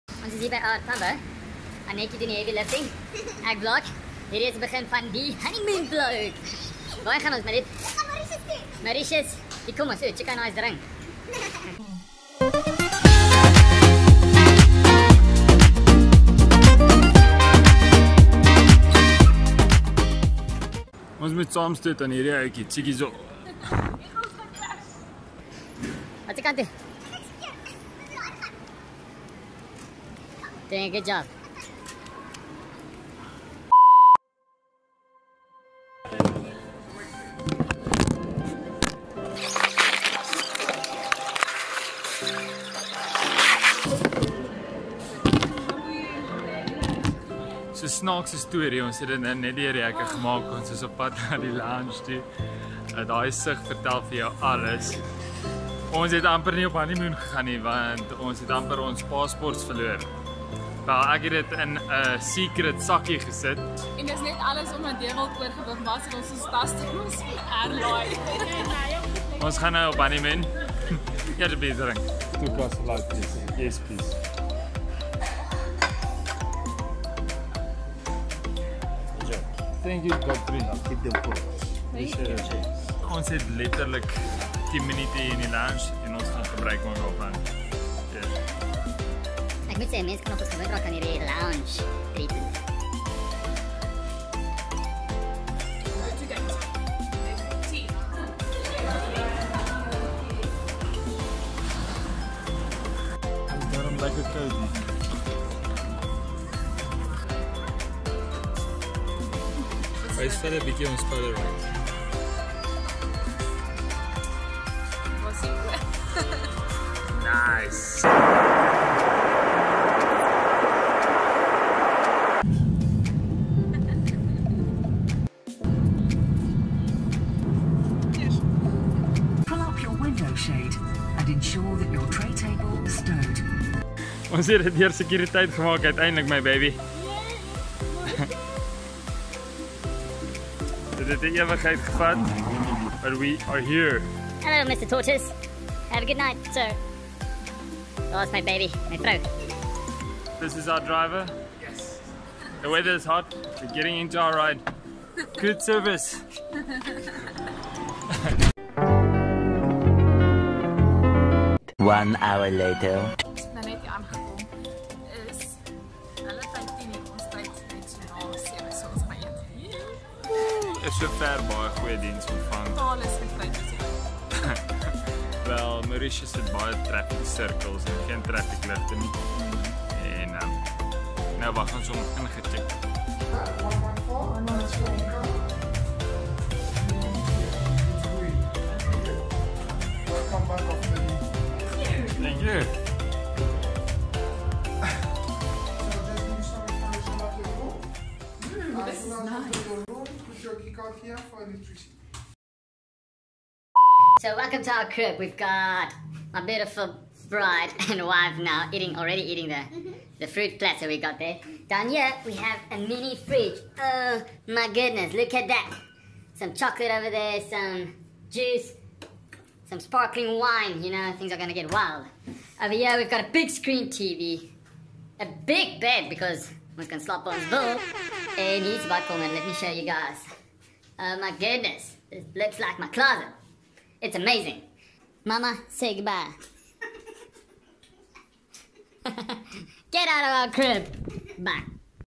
Honeymoon Vlog 1 - Mauritius 🇲🇺
Today is Valentine's day 2020 and what better day than today to upload the 1st video of our amazing honeymoon last year September.

Baie lief vir hierdie vrou van my. ❤️

Happy 5 months of being married. 🎉